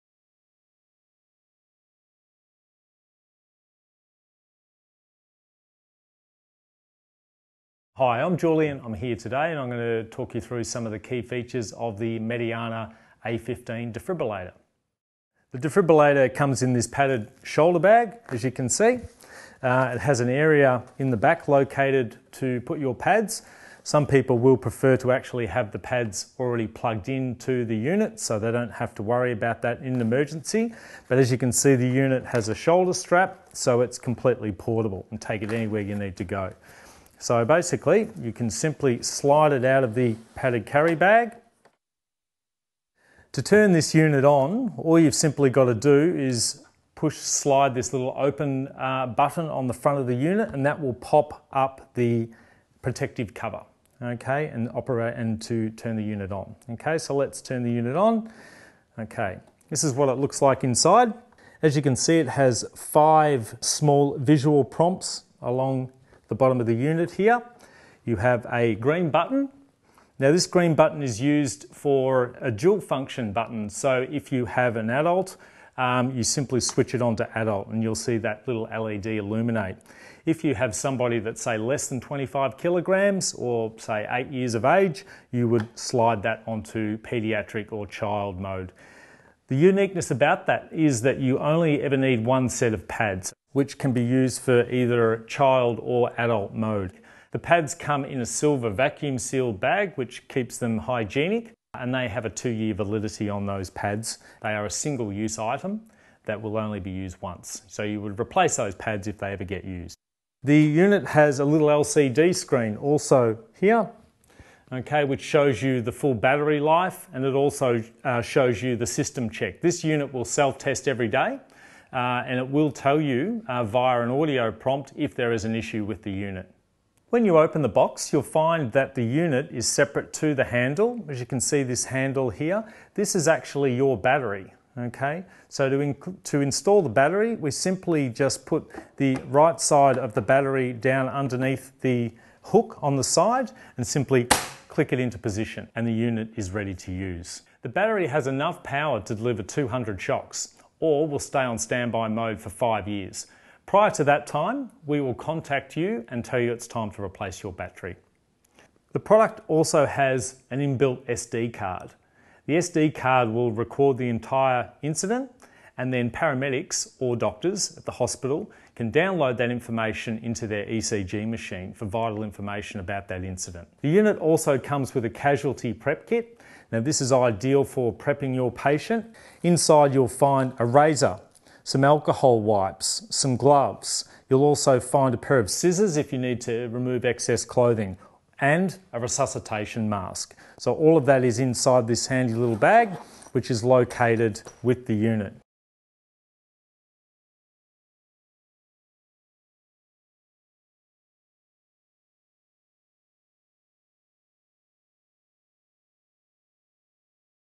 Features of the Mediana A15 defibrillator
ELAA & Australian First Aid have teamed up to give early learning services a fantastic deal on high quality defibrillator kit that could save the life of a child or adult. Purchase the Mediana A15 through the ELAA website & save over $500 on the AFA RRP